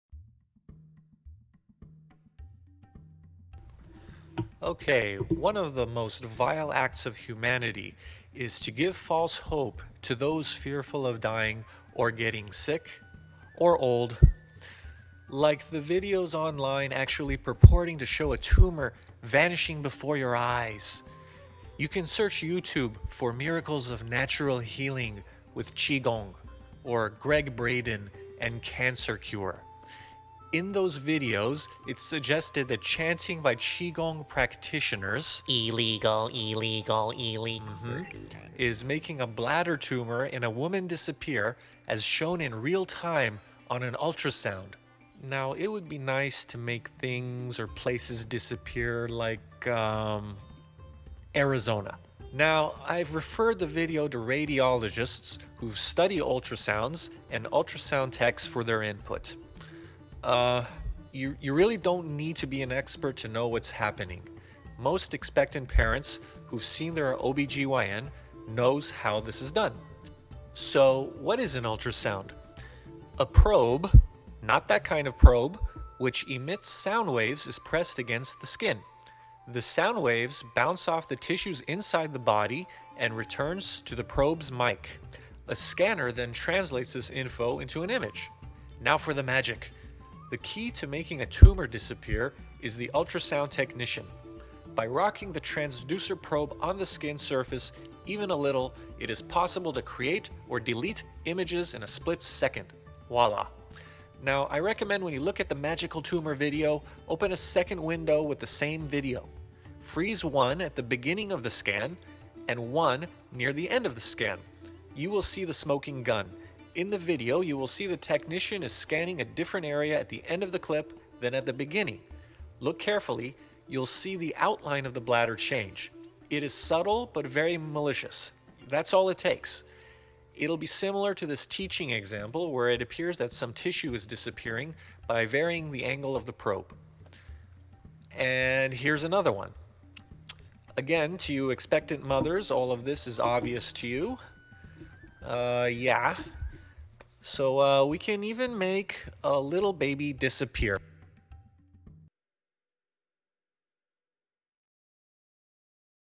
Science of Miracles Cult Fraud Tumor Dissolves
Bladder tumors are highly curable when caught early. Do not go to a "qigong master" as primary treatment.   I do not want to hear of anyone dying a horrible death where the cancer devours the body as it spreads.   If you have an open mind, read on...

Why do people believe this? Ultrasound manipulation.  (Expand description).
You will have to search the actual video of "tumor disolves"  yourself.  I won't play the actual video here. I just analyze the video.

For those of you not understanding what your own eyes are seeing...Imagine watching a sunset. You believe the sun is going down and disappearing. What you are really experience, is the earth's rotation. You do not need to see the earth rotating. You know it is because of  changing shadows, position of stars, etc. This is the same thing in the video. How do we know the technician is changing the position of his probe? The outline of the bladder is changing just like shadows and stars...

The smoking gun in the video is at 2:12 (of my video).  That frame is from the exact same video from Asia that purports to claim a tumor is dissolving.  The frame on the left shows a bladder tumor. The frame on the right shows the same bladder from a different angle.  It is used here and elsewhere as a teaching example to demonstrate fraud. I've contacted several prominent radiologists who are now aware of this fraud and are working out a plan to combat this crime against the sick.

The fact is that 94% of bladder cancers are curable (5yr survival rate) when diagnosed early . Using chanting alone will kill you.  There is no statistic showing any survival from chanting.  Nor has there ever been a randomized  clinical study performed to see if chanting works. (Frauds know it will kill you...they don't care).
This is not a video in support of, or against traditional or eastern medicine. This is a video against FRAUD. Try to use logic and reason in your comments. Qigong is appropriate for relaxation only, which may ease recovery after traditional treatment.